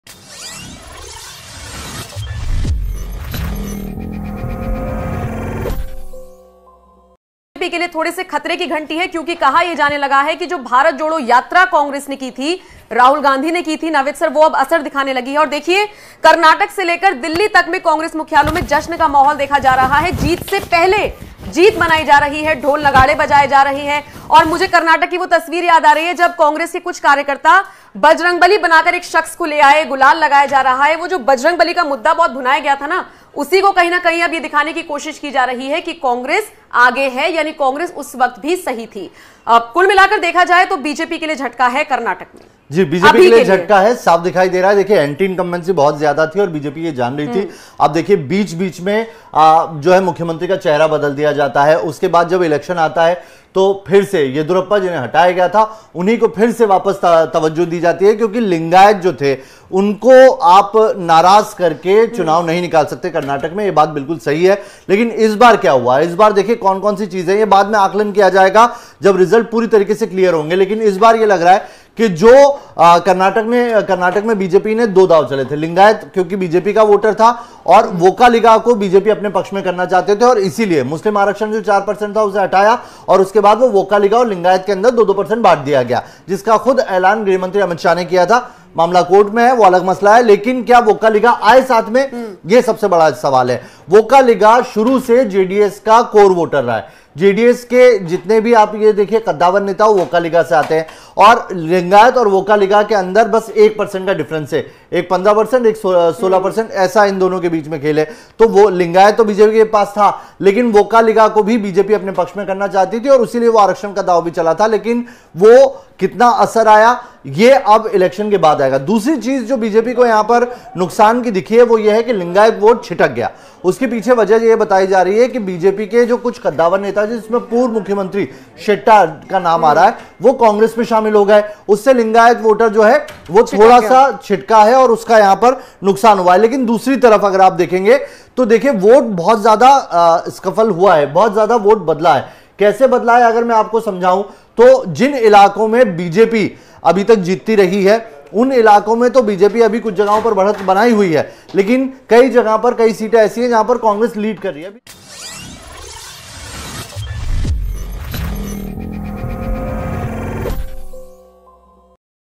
Karnataka में Congress के जीतते ही लगने लगे बजरंग बली के जयकारे !
ashu